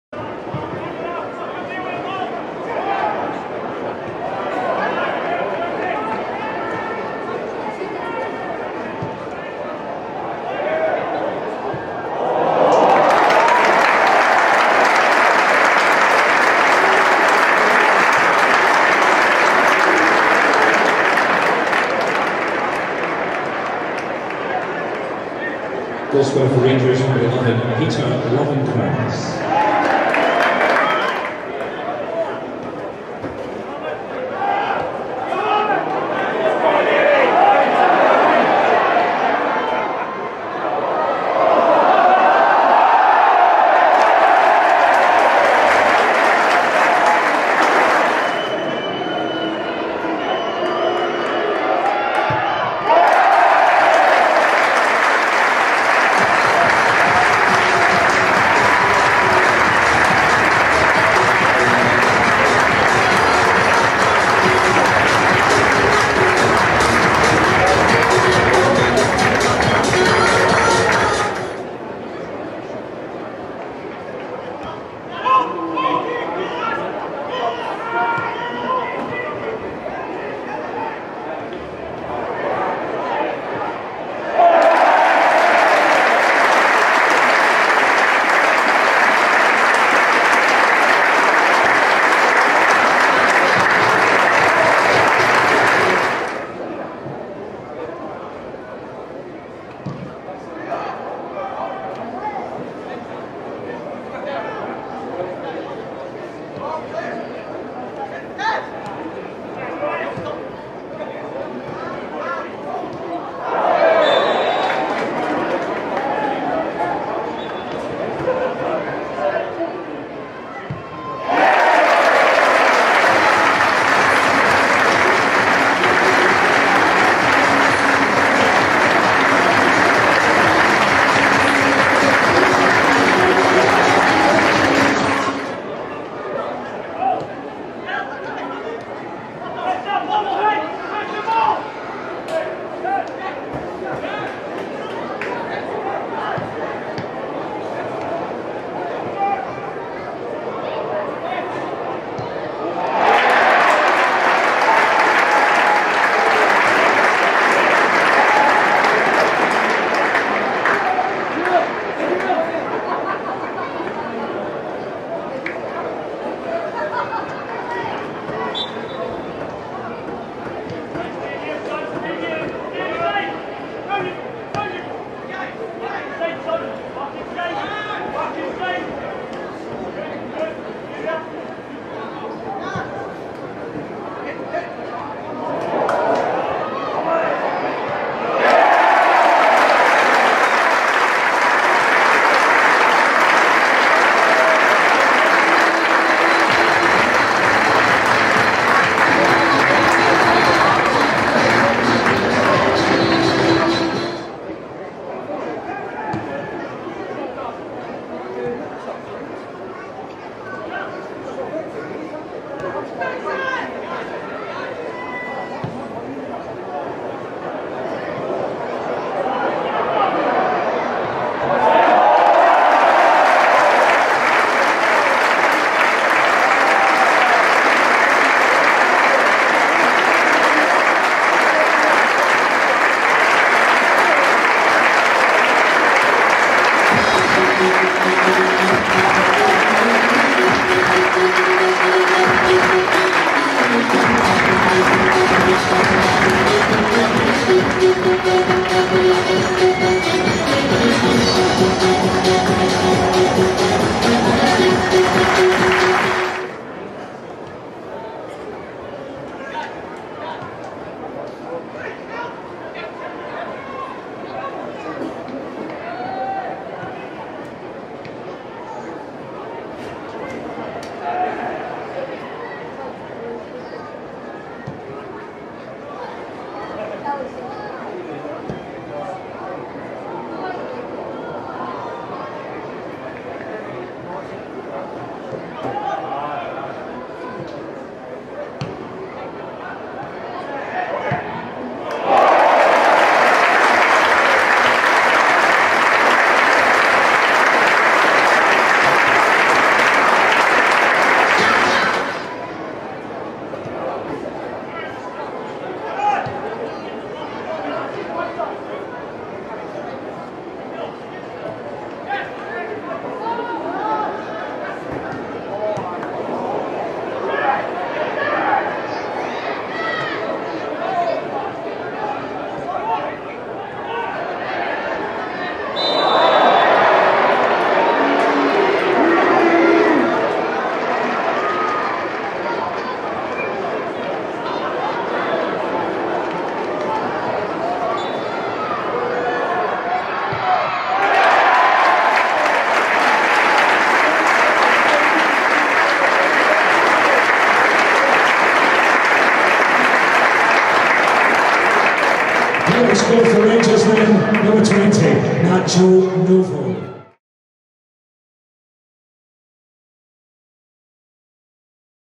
Just The Goals | Kris Boyd Testimonial
Catch Kris Boyd's hat-trick and a cracking strike from Paul Di Giacomo.